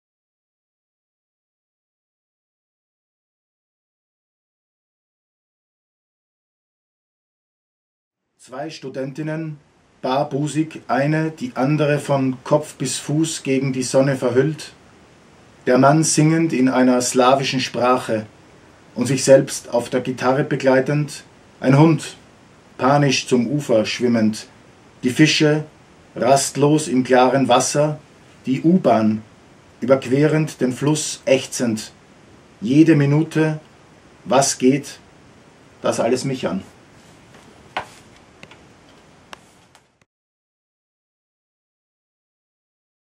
Wolfgang Kindermann: "Neue Donau 17:05"
Wolfgang Kindermann las das Gedicht "Neue Donau 17:05". Buchpräsentation: Wolfgang Kindermann "Schon ist es falsch" - Eduition FZA - read!!ing room, 07. Oktober 2016